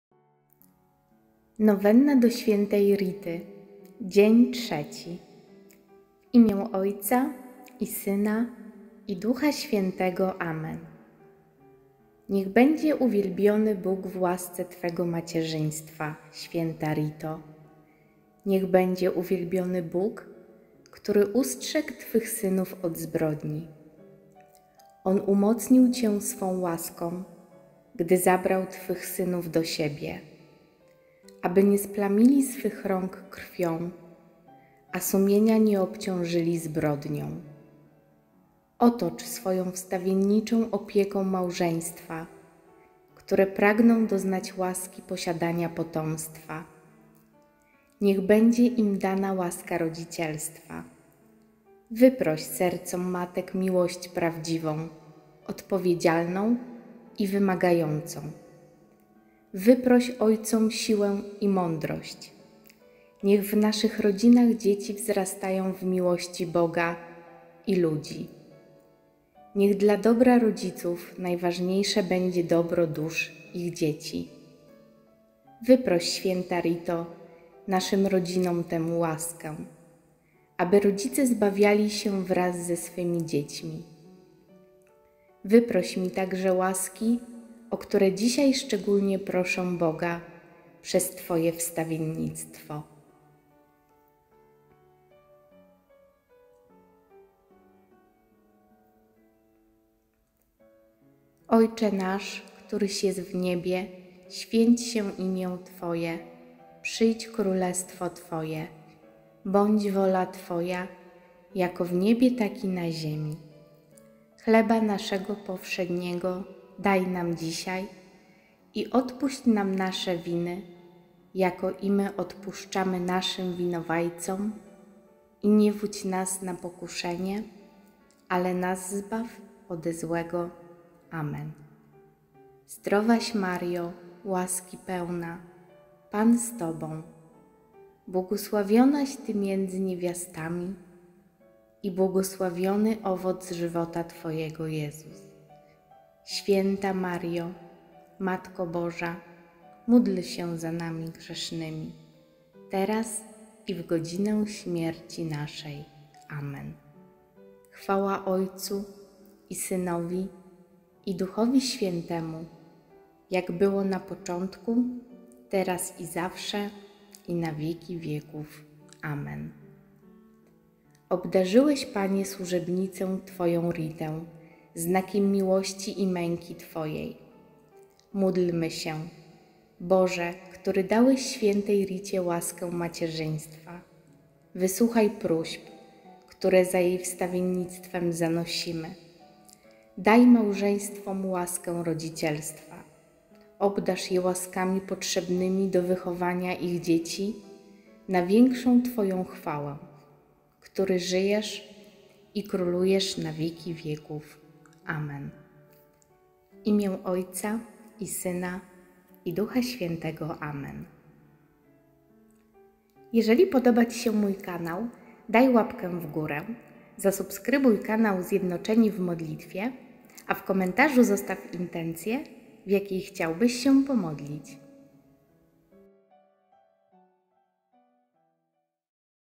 Nowenna do Świętej Rity - dzień trzeci
Nowennę do św. Rity odmawiamy od 13 do 22 dnia każdego miesiąca.
Nowenna do Św. Rity składa się z 9 dni, każdego dnia odmawiamy krótką modlitwę, którą kończy wezwanie o wstawiennictwo Św. Rity przed Bogiem w naszej indywidualnej sprawie oraz modlitwa Ojcze nasz, Zdrowaś Maryjo, Chwała Ojcu. Nowennę można odprawiać w dowolnym czasie przez cały rok, jeżeli tylko czujemy taką potrzebę.